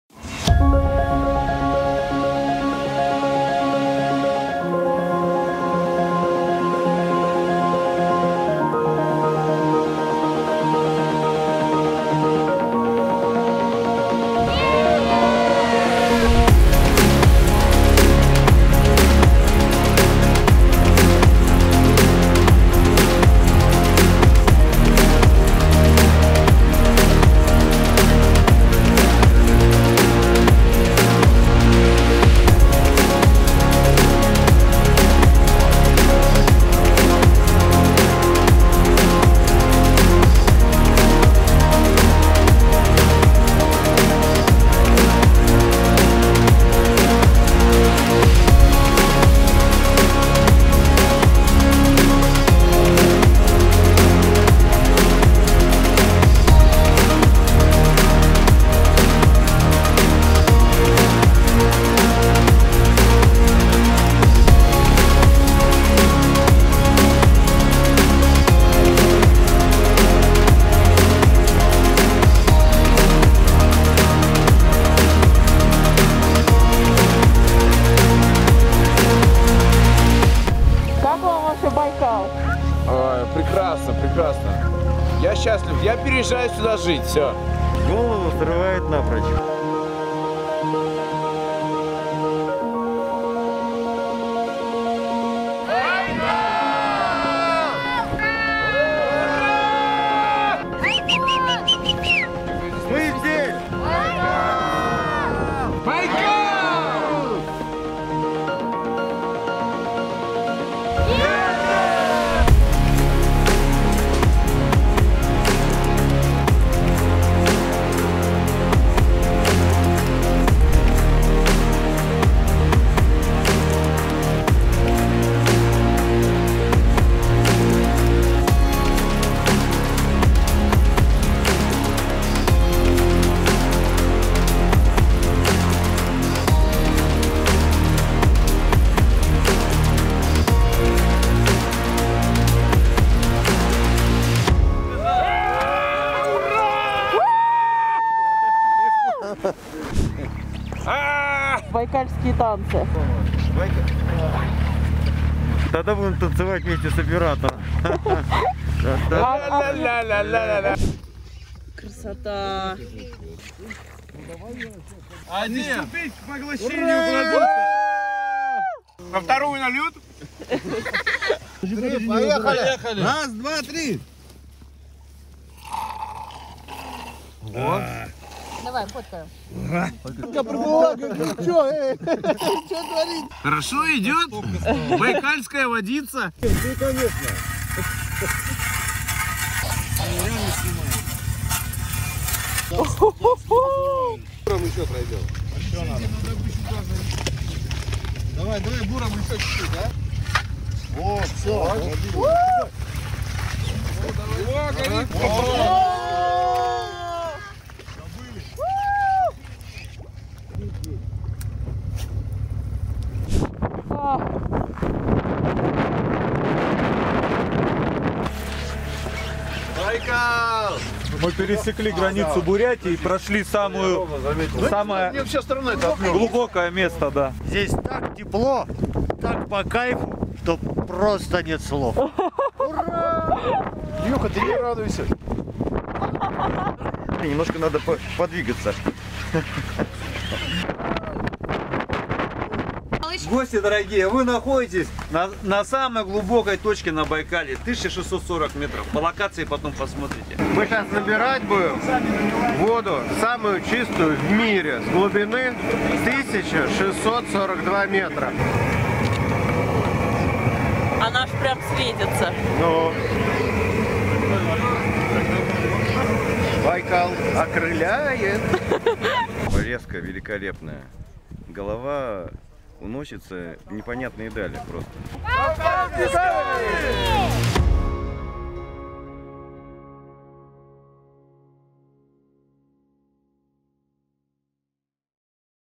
Экспедиция на Байкал вместе с клубом BRP VETERR
Экспедиция на зимний Байкал вместе с клубом BRP VETERR

Февраль 2021 года

Невероятный красивый лед, мороз, багги, квадроциклы и классная компания!

Съемка и монтаж: @liselesok